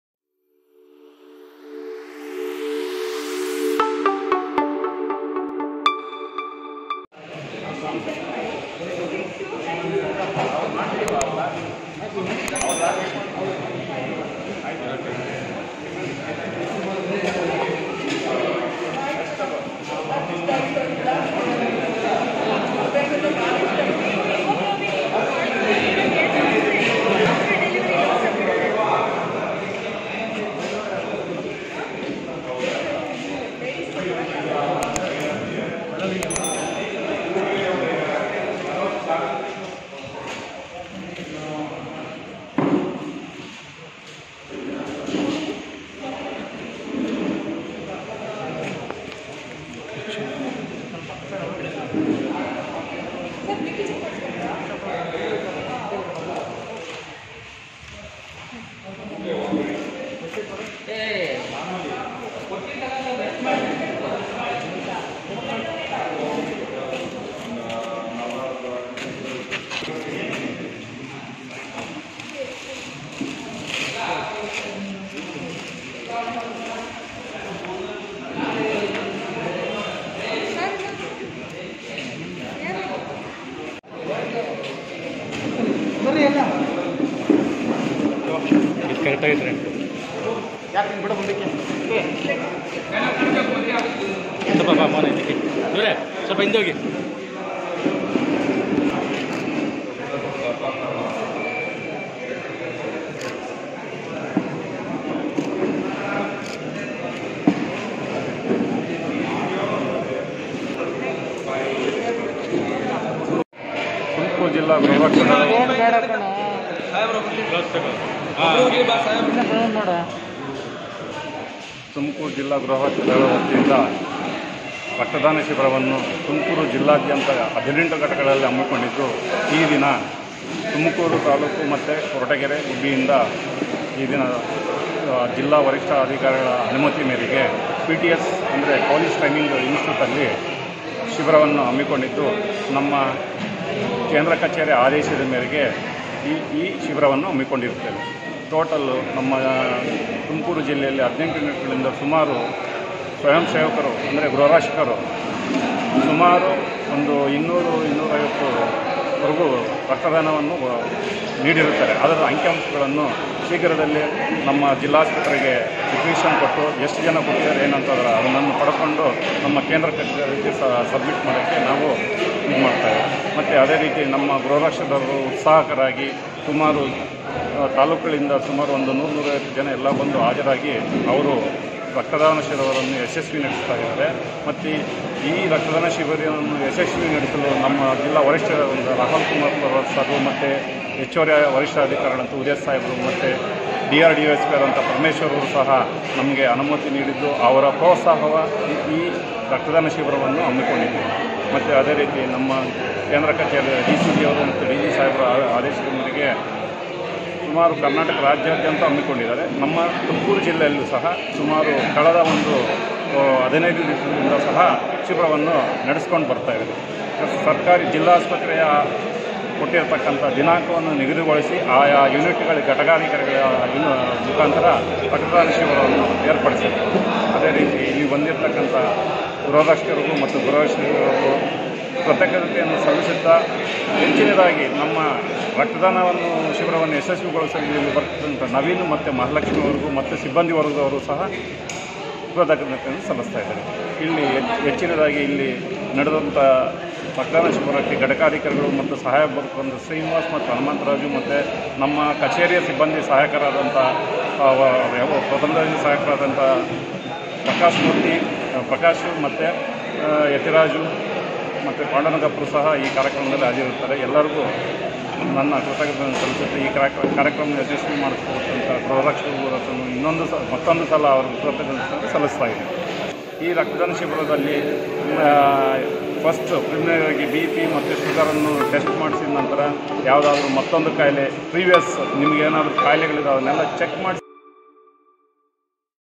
ಗೃಹರಕ್ಷಕ ದಳದಿಂದ ರಕ್ತದಾನ ಶಿಬಿರ...
ತುಮಕೂರಿನಲ್ಲಿ ಗೃಹರಕ್ಷಕದಳದ ವತಿಯಿಂದ ಸ್ವಯಂ ಪ್ರೇರಿತ ರಕ್ತದಾನ ಶಿಬಿರ ನಡೆಸಲಾಯಿತು.